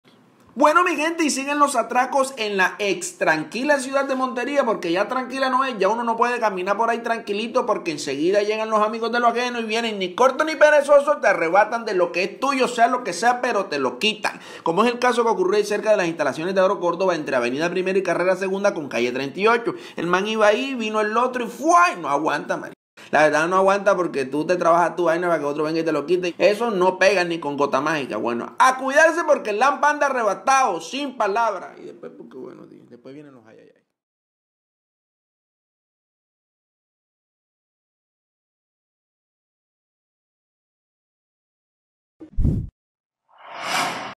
Siguen los atracos en la ciudad de Montería, como el caso que ocurrió cerca de las instalaciones
GenteGs | Siguen los atracos en la ciudad de Montería, como el caso que ocurrió cerca de las instalaciones de Agrocórdoba entre carrera 1 y 2 con calle 38.